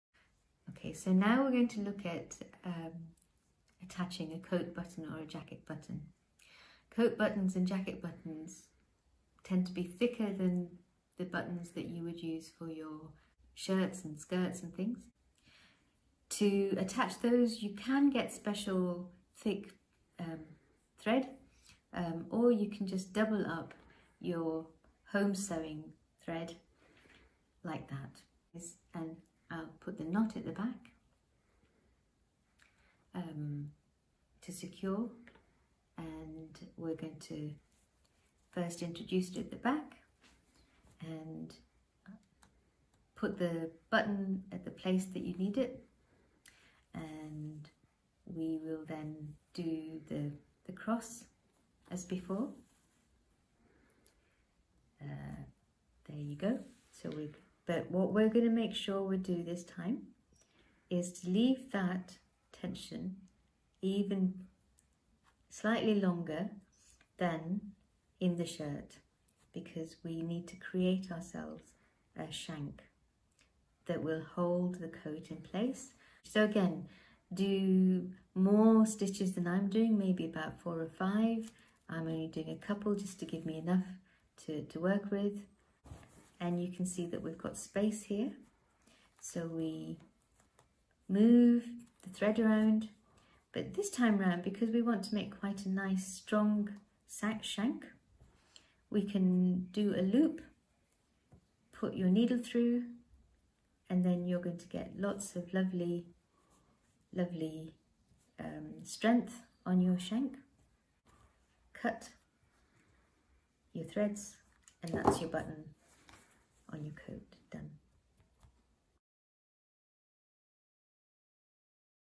Sewing a coat button
Needle and thread at the ready… Dr Pammi Sinha from the University of Leeds School of Design is here to show you how to mend and repair clothes with some very simple techniques.

This video has been produced for BeCurious20! With live family events, fun activities, challenges and inspiring evening talks… there’s something for everyone!
and follow the fun on Twitter with the BeCurious20 hashtag.